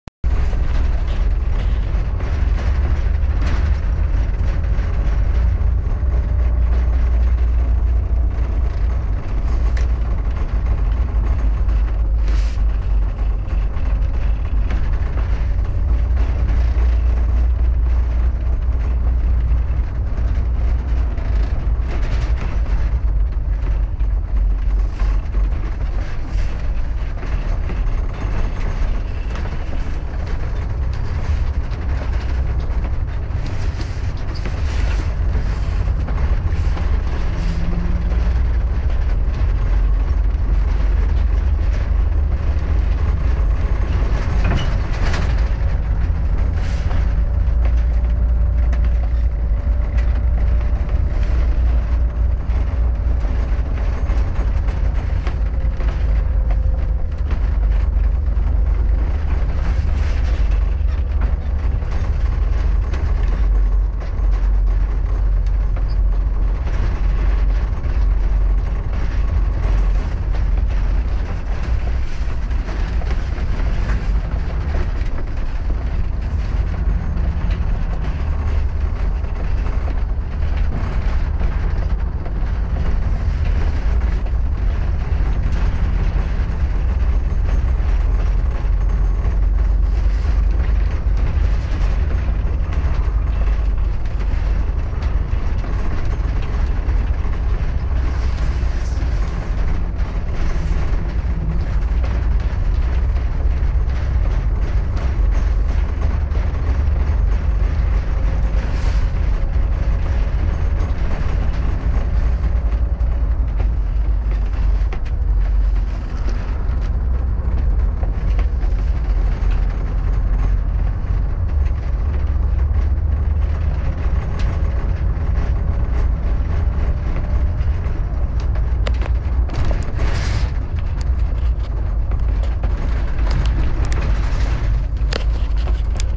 Neve Ponte alla piera
Percorso da Tavernelle a Pieve Santo Stefano via Ponte alla Piera Guarda tutti i video degli utenti di ilMeteo.it